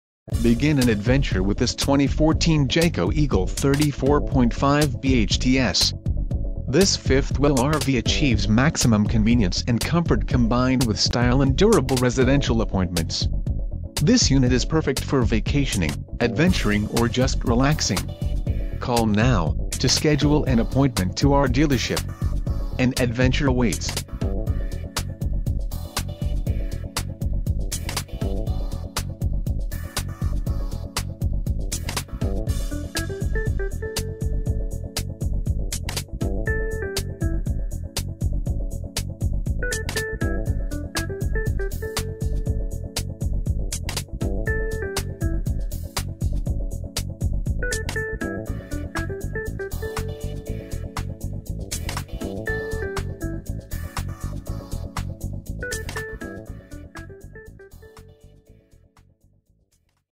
2014 Jayco Eagle - Colerain RV - OH, IN 45251
Brownstone interior decor and triple slide outs make this new fifth wheel in bunkhouse a terrific choice. You\'ll find this Jayco fifth wheel to be very appealing. This 2014 Eagle 34.5BHTS has a ceiling fan over the kitchen and is furnished with a dinette and a versatile hide a bed sofa. This 34 foot long bunkhouse has multiple sleep accommodations and a large awning. , Customer Value Package:\r\nCentral Air Conditioner,\r\n16\" Tire w/Aluminum Rim,\r\n32\" HD LED TV,\r\nCD/DVD MP 3 Input Jks Snd Sys,\r\nElectric Patio Awning,\r\nMicrowave,\r\nOutside Shower,\r\nOversized 21\" Oven,\r\nPower Vent in Bathroom,\r\nTile Look Tub/Shower Surround,\r\nWater Heater Bypass,\r\n\r\nOptions Included With This Unit:\r\n\r\nHard Surface Countertop Kitchen,\r\nKeyless - Main Entry Door,\r\nFBG Hi Gloss Ivory Sidewall,